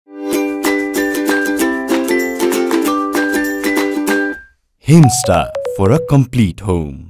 Stay Cool, Stay Comfortable | Experience comfortable space with Himstar Air Conditioner
Experience the pleasure of comfortable space with Himstar Air Conditioner. Equipped with  Sleep Mode and 24h Timer, Auto-Protection and Self-Diagnosis features it ensures perfect weather for your home and creates a relaxing atmosphere in your home. It comes with 1 year full warranty, 3 years compressor warranty and free installation.